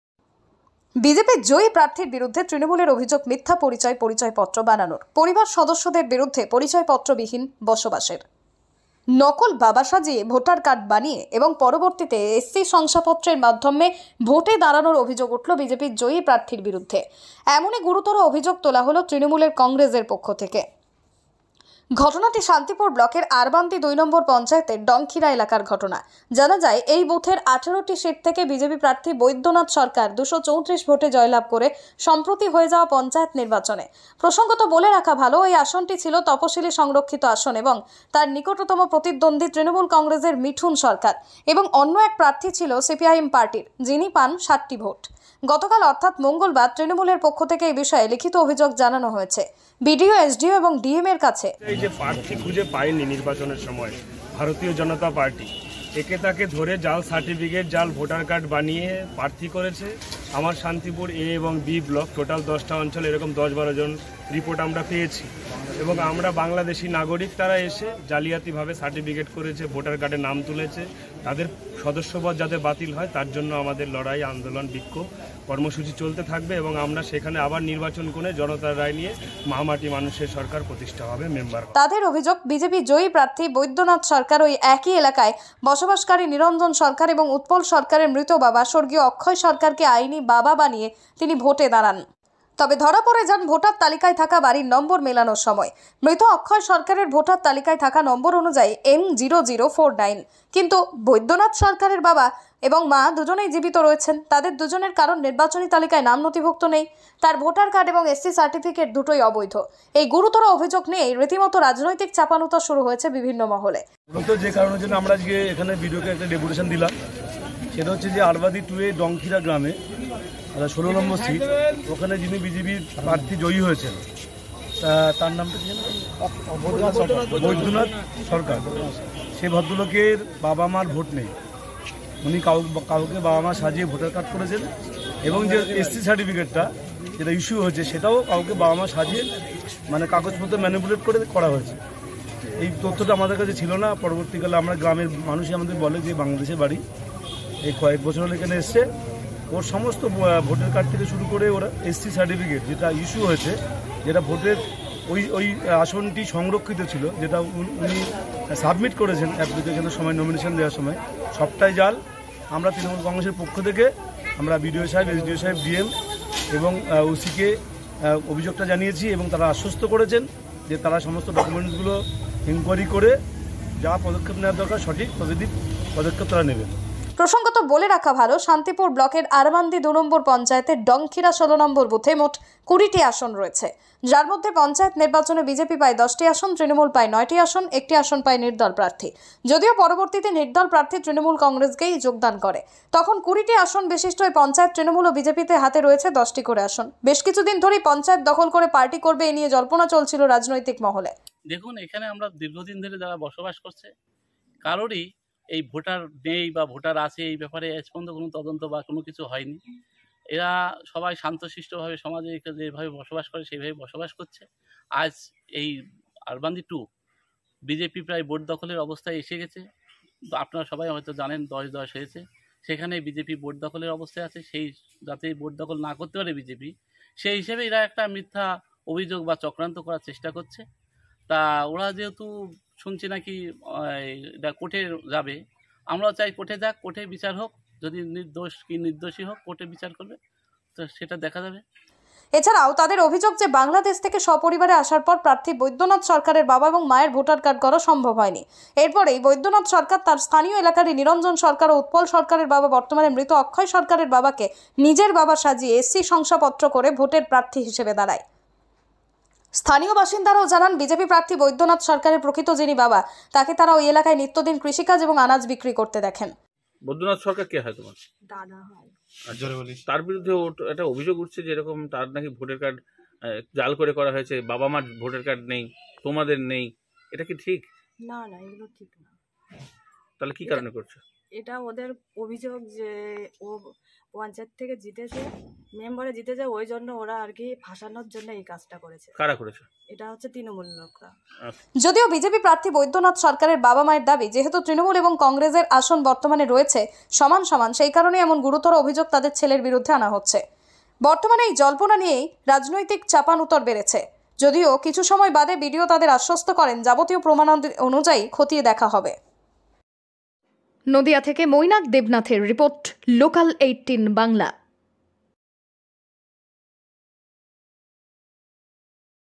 Panchayat Election 2023: বাবা, কিন্তু আসল নয়! নকল নজরে পড়তেই কঠিন শাস্তির মুখে বিজেপির জয়ী প্রার্থী
শান্তিপুর: নকল বাবা সাজিয়ে ভোটার কার্ড বানিয়ে এবং পরবর্তীতে SC শংসাপত্রের মাধ্যমে ভোটে দাঁড়ানোর অভিযোগ উঠল বিজেপির জয়ী প্রার্থীর বিরুদ্ধে। এমনই গুরুতর অভিযোগ তোলা হল তৃণমূলের পক্ষ থেকে। ঘটনাটি শান্তিপুর ব্লকের আরবান্দি দুই নম্বর পঞ্চায়েতের ডংক্ষিরা এলাকার ঘটনা।